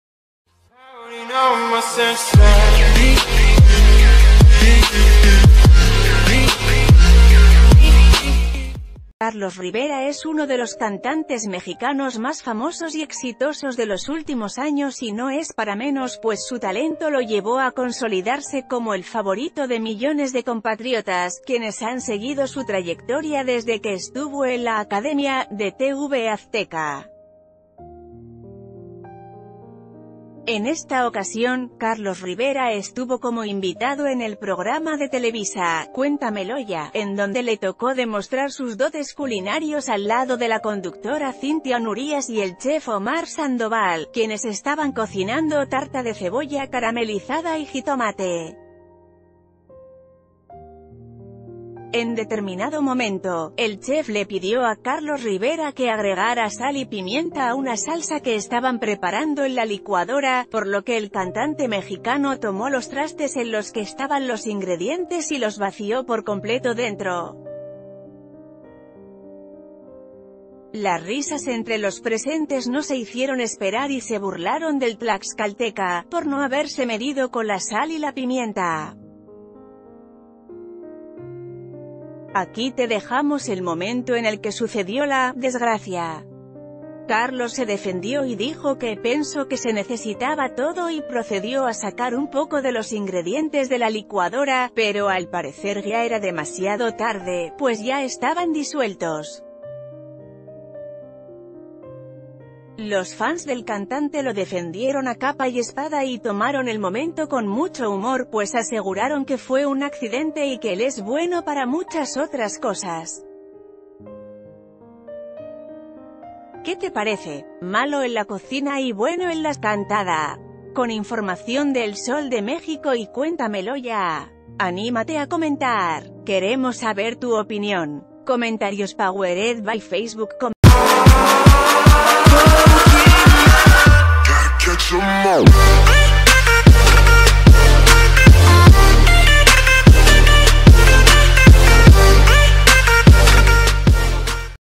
Difunden ridículo de Carlos Rivera intentando cocinar en programa de Televisa
Carlos Rivera es uno de los cantantes mexicanos más famosos y exitosos de los últimos años y no es para menos, pues su talento lo llevó a consolidarse como el favorito de millones de compatriotas, quienes han seguido su trayectoria desde que estuvo en La Academia, de TV Azteca. En esta ocasión, Carl...